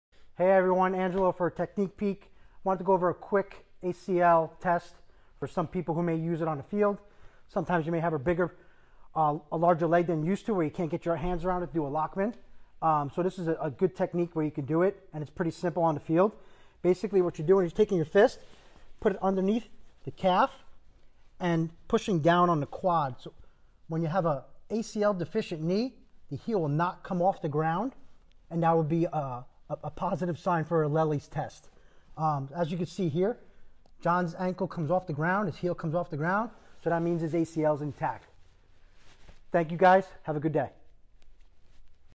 Alternate ACL Tear Test (Lelli's Test) | Technique Peek Series
This Technique Peek Series Video features Angelo Marsella, MA, ATC, USAW demonstrating how to perform the Lelli's Test used to assess if an athlete has a torn ACL. This technique is fairly simple and can come in handy when dealing with larger athletes.